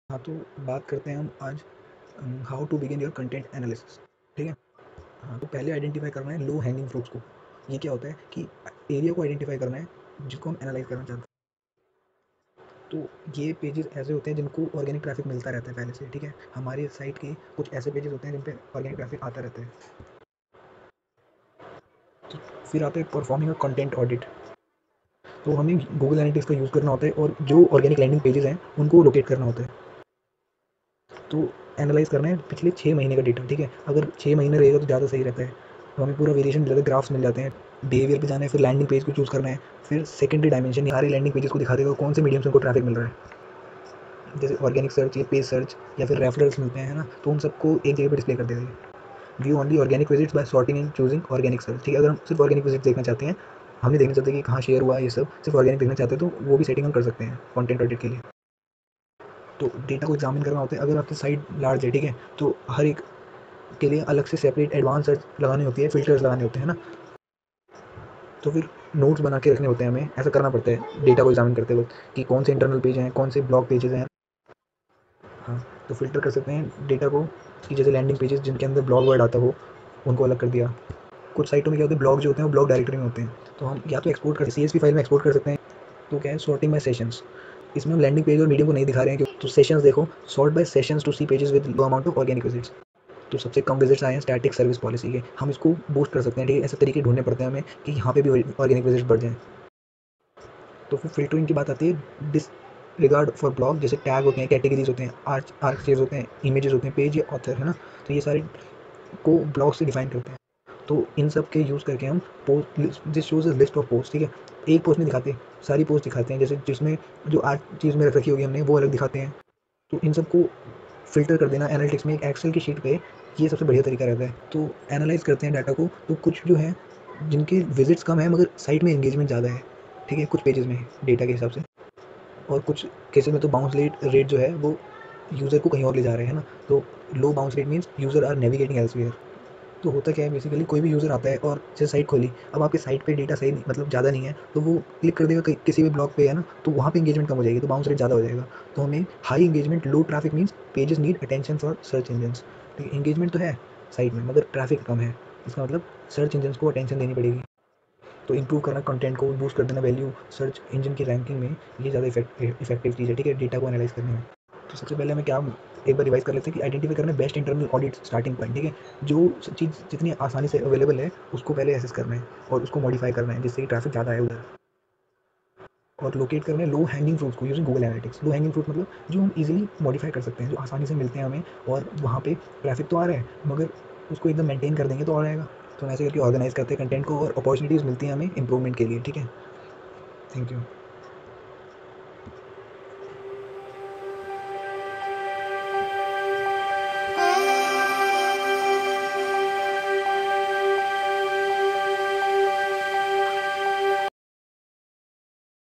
HOW TO BEGIN YOUR CONTENT ANALYSIS | Content's Organic Search using Google Analytics | TECH_ED
Note : The visuals in this video are not mine solely these are from UC Davis Ext. for the purpose of understanding.